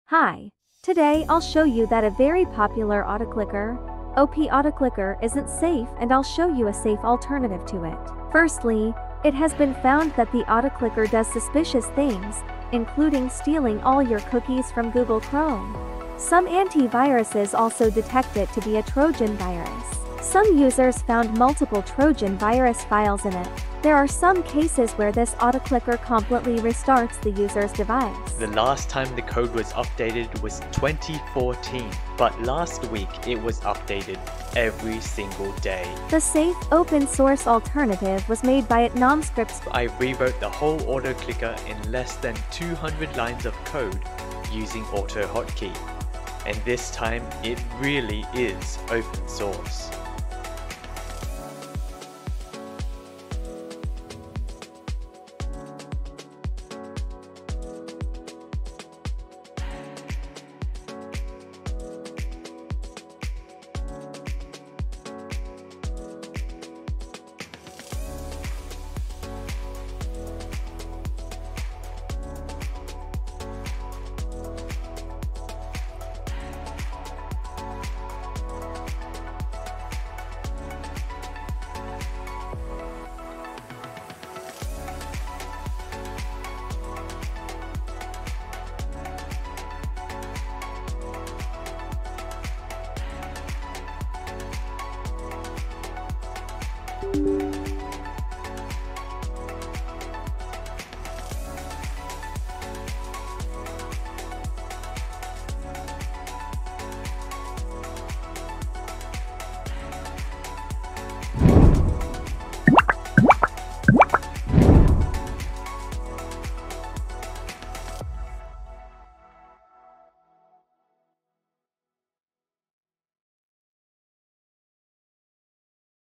👿 OP Autoclicker is a virus, here's why and what are it's safe alternatives
➤⠀Shout-outs to the VIP members of my community server : Mechaniclad!

auto, autoclick, autoclicker, virus, trojan, exposed